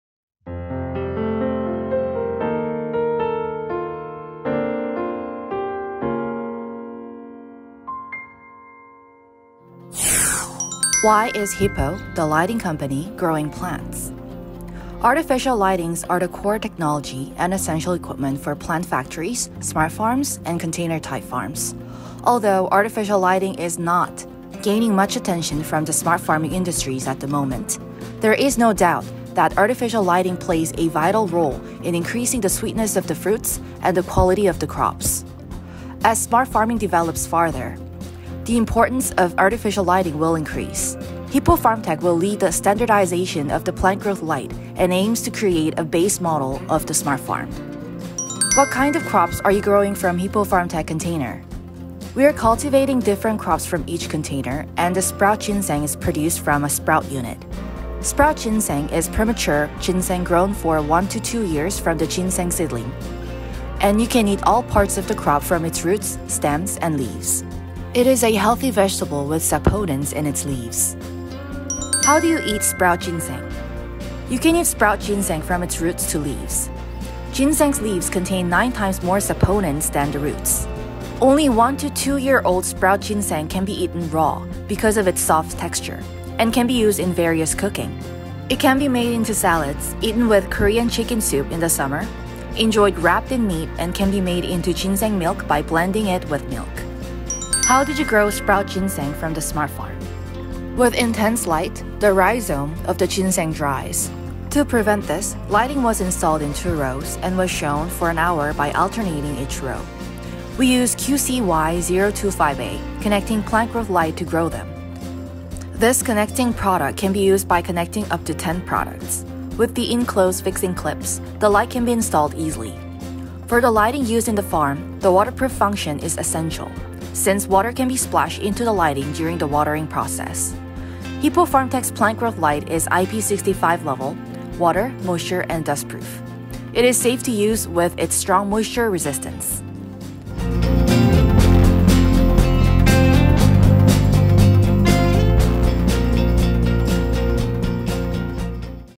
HIPPO Smart Farm & Fresh ginseng | Hippo Farmtech
Sprout ginseng can be used in various ways as salad, juice, and vegetables because you can eat the whole ginseng from roots to stems and leaves.
Also, anyone can eat it conveniently.
In particular, the saponi content of the whole sprout ginseng is known to be similar to that of regular ginseng.
In addition, studies have shown that the leaves and stems of sprout ginseng grown as hydroponic vegetables are rich in anti-inflammatory and whitening effects.